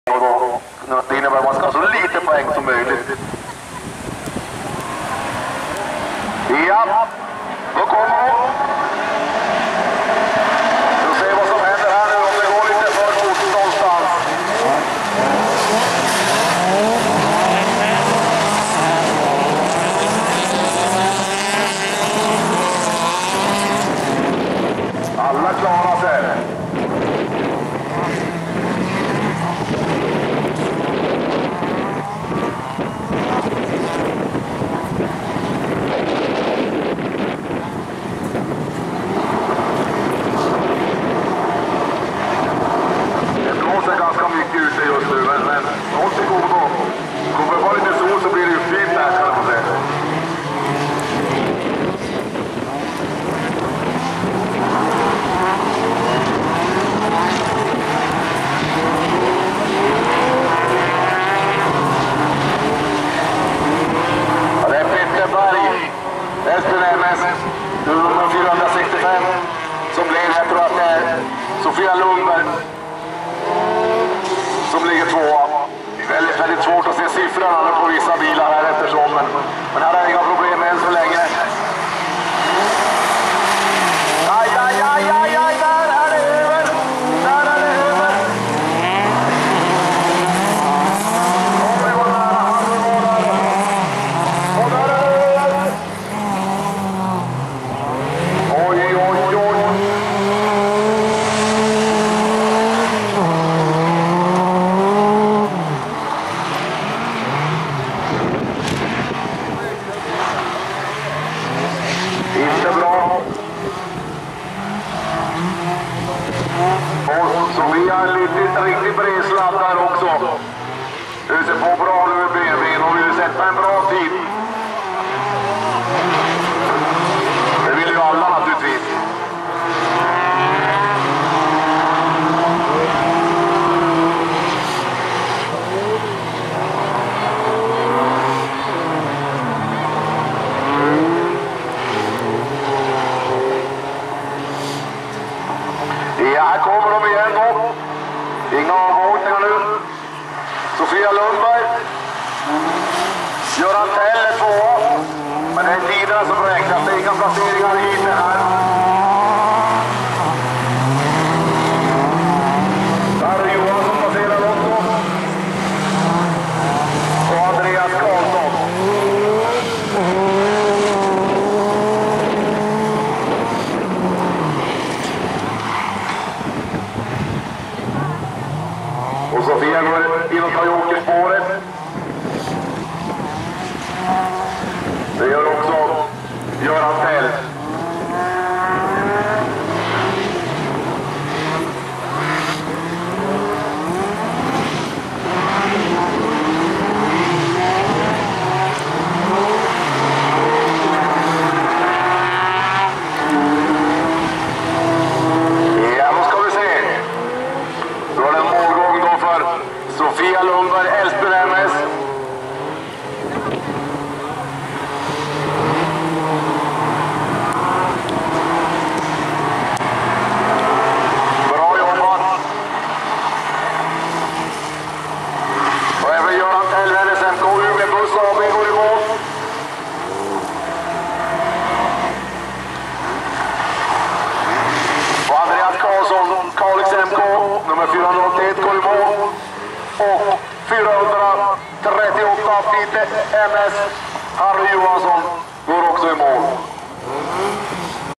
Nordic Cup 2016 - Älvsbyn - Q2 - Läktare
Nordic Cup 2016
Höghedenbanan, Älvsbyn

Göran Tell i kvalomgång 2 i RC klass 4, max 2150 cc

Filmat med en Sony Handycam HDR-PJ410